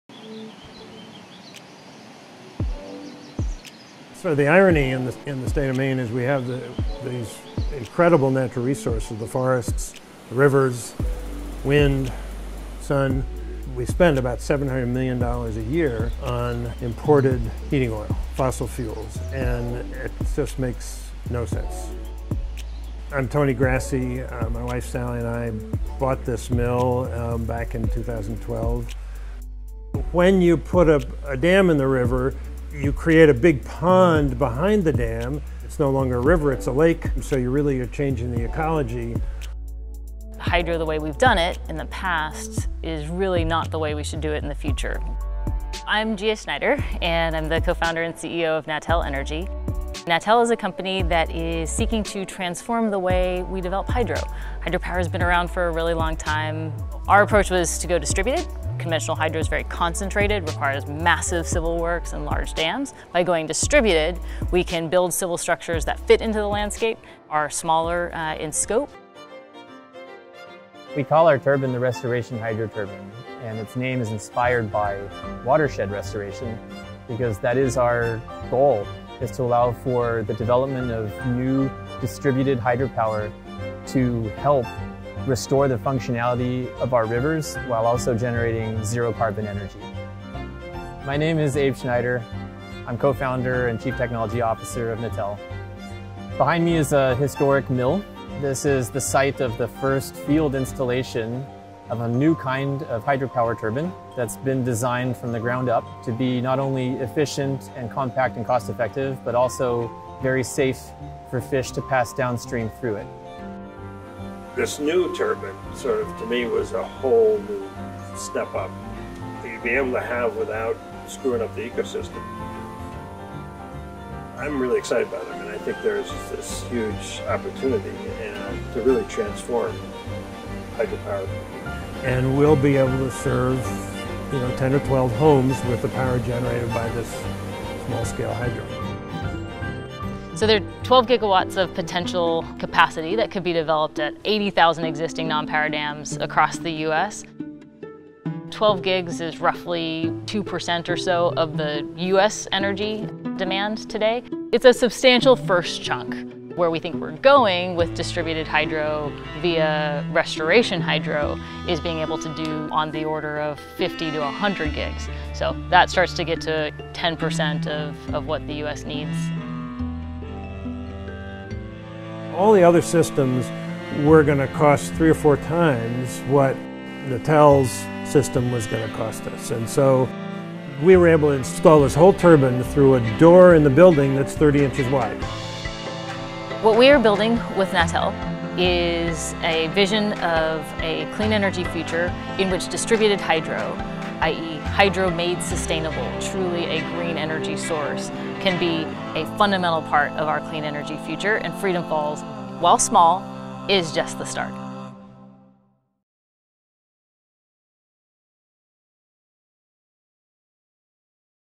Transforming Our Energy Landscape: The FishSafe™ Restoration Hydro Turbine
In 2019 Tony Grassi, owner of The Mill at Freedom Falls, partnered with Natel to install the first FishSafe™ Restoration Hydro Turbine, retrofitting a once-purposeless sawmill with enough power to meet the needs of the small community of Freedom, Maine.

Like Tony's mill, there are hundreds of existing small dams in the United States that are no longer being used as a power source or were constructed for non-power purposes. By retrofitting them with FishSafe™ hydro turbines, Natel is working with communities to leverage the infrastructure they already have to generate new, clean energy that could deliver close to 10 percent of the country's energy demand, while restoring safe downstream fish passage.

*Video narration:* Tony Grassi, Gia Schneider, Abe Schneider
*Filming:* Kate Stirr and Andy Baumgartner
*Animations:* Emerson Collective
*Video edit:* Ian Colon